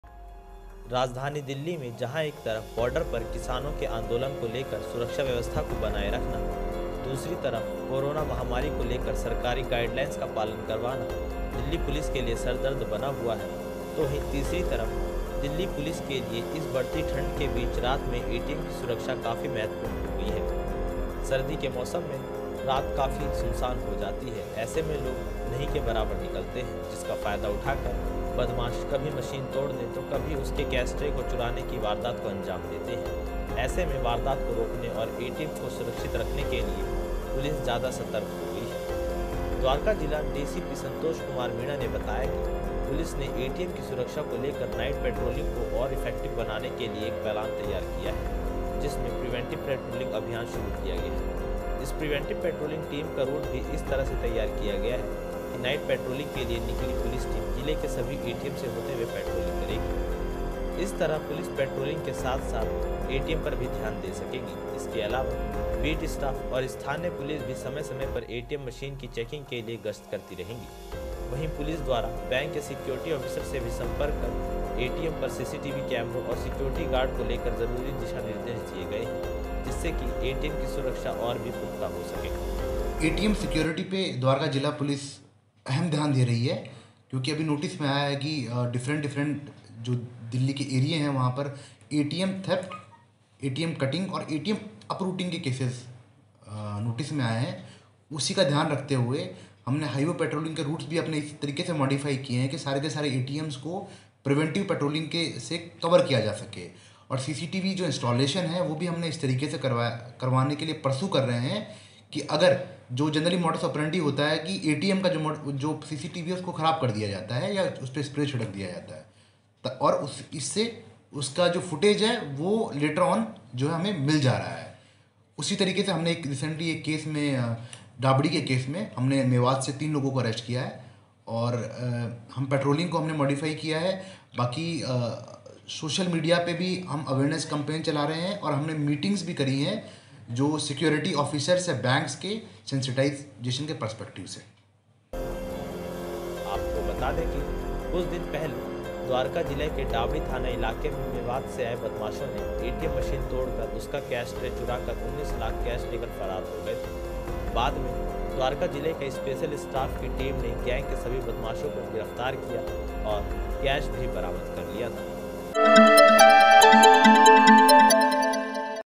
सर्दी में सरदर्द बने एटीएम के लिए नाइट पेट्रोलिंग
राजधानी दिल्ली में जहां एक तरफ बॉर्डर पर किसानों का आंदोलन को लेकर सुरक्षा व्यवस्था को बनाए रखना, दूसरी तरफ कोरोना महामारी को लेकर सरकारी गाइडलाइन का पालन करवाना दिल्ली पुलिस के लिए सिरदर्द बना हुआ है. तो वहीं तीसरी तरफ दिल्ली पुलिस के लिए इस बढ़ती ठंड के बीच रात में एटीएम की सुरक्षा काफी महत्वपूर्ण हो गयी है. सर्दी के मौसम में रात काफी सुनसान हो जाती है और ऐसे में लोग नही के बराबर निकलते हैं। जिसका फायदा उठाकर बदमाश कभी मशीन तोड़ने, तो कभी उसके कैश ट्रे को चुराने की वारदात को अंजाम देते हैं. ऐसे में वारदात रोकने और एटीएम को सुरक्षित रखने के लिए पुलिस ज्यादा सतर्क हो गई है शुरू किया प्रिवेंटिव पेट्रोलिंग अभियान,,द्वारका जिला डीसीपी सन्तोष मीणा ने बताया की पुलिस ने एटीएम की सुरक्षा को लेकर नाइट पेट्रोलिंग को और इफेक्टिव बनाने के लिए एक प्लान तैयार किया है. जिसमें प्रीवेंटिव पेट्रोलिंग अभियान शुरू किया गया है. इस प्रिवेंटिव पेट्रोलिंग टीम का रूट भी इस तरह से तैयार किया गया है, की नाइट पेट्रोलिंग के लिए निकली पुलिस टीम जिले के सभी एटीएम से होते हुए पेट्रोलिंग करेगी.

पेट्रोलिंग के साथ साथ एटीएम पर होगी नजर,,

इस तरह पुलिस, पेट्रोलिंग के साथ-साथ एटीएम पर भी ध्यान दे सकेगी. इसके अलावा बीट स्टाफ और स्थानीय पुलिस भी समय-समय पर एटीएम मशीन की चेकिंग के लिए गश्त करती रहेगी. वही पुलिस द्वारा बैंक के सिक्योरिटी ऑफिसर से भी सम्पर्क कर एटीएम पर सीसीटीवी कैमरों और सिक्योरिटी गार्ड को लेकर जरूरी निर्देश दिए हैं. जिससे की एटीएम की सुरक्षा और भी पुख्ता हो सके.स्पेशल स्टाफ ने गिरफ्तार कर कैश किया बरामद,,आपको बता दें कि कुछ दिन पहले द्वारका जिला के डाबड़ी थाना इलाके में मेवात से आये बदमाशों ने एटीएम मशीन तोड़कर उसका कैश ट्रे चुराकर 19 लाख कैश लेकर फरार हो गए थे. बाद में द्वारका जिला के स्पेशल स्टाफ की पुलिस ने गैंग के बदमाशों को ना सिर्फ गिरफ्तार किया ब्लकि कैश भी बरामद किया था.